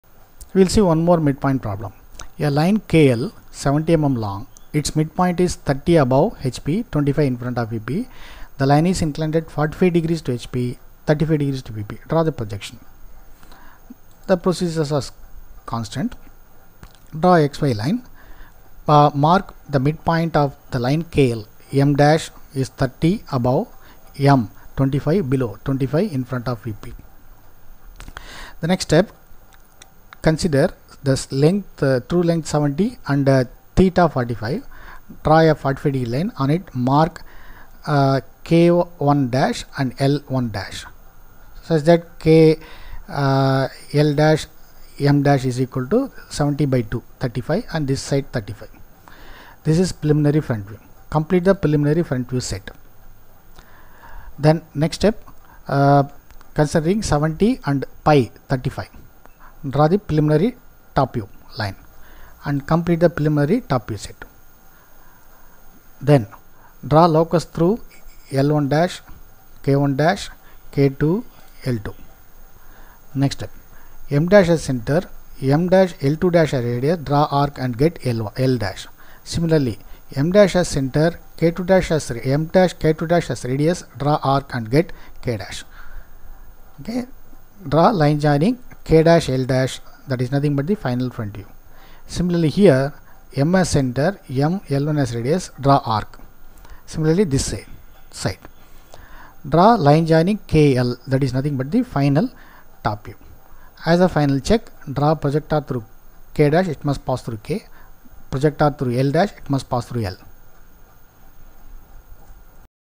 8.24 Projection of Straight Lines Mid Point Problem
Projections of Straight Lines :: Mid Point Problem:-
Problem #25: A line KL 70mm long. Its mid point is 30mm above HP and 25mm infront of VP. If the line is inclined at 45° to the HP and 35° to VP draw the projection